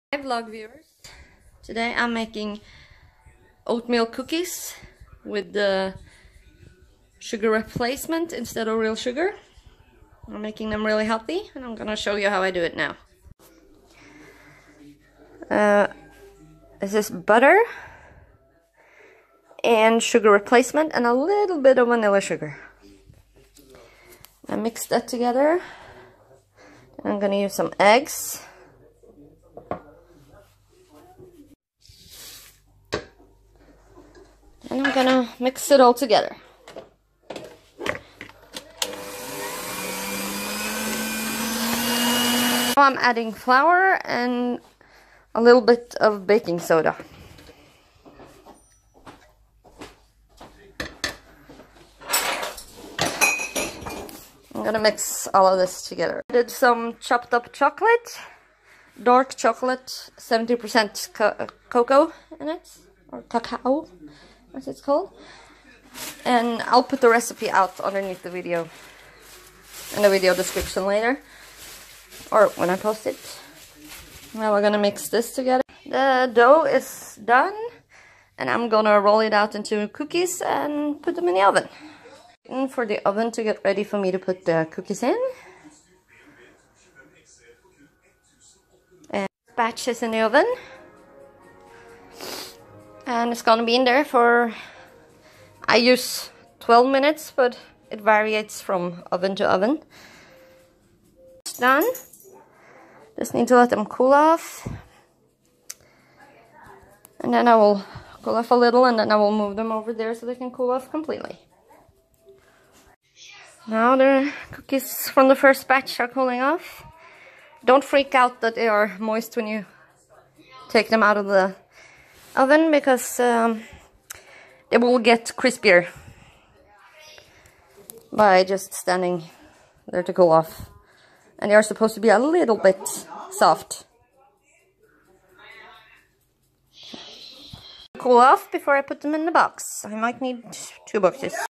Tiana's vlog: How to make healthy cookies that tastes amazing.
I originally found the recipe on a Norwegian blog but I changed it a bit to make it better and to make it "my own".

Recipe:
Oatmealcookies with sugarreplacement:

200grams of roomtempered butter/margarine.

150grams of sugarreplacement.

2 desiliters of oats.

2 eggs.

125grams wheat flour.

1 teaspoon of baking soda.

1 teaspoon of vanillasugar.

100-200grams of dark chocolate(70% cocoa)(I use 100grams).

Whip butter/margarine and sugarreplacement til it's airy. Then add the eggs, vanillasugar and oats.

Chop the chocolate.

Mix the wheat flour and baking soda into the dough, then add the chopped chocolate and mix it into the dough.

Divide the dough into pieces(8-20 pieces,depending on how big you want the cookies to be). You can press down on the pieces with a spoon if they get to thick.

Putt the batch in the middle of the oven for about 10-12minutes at 180degrees celsius.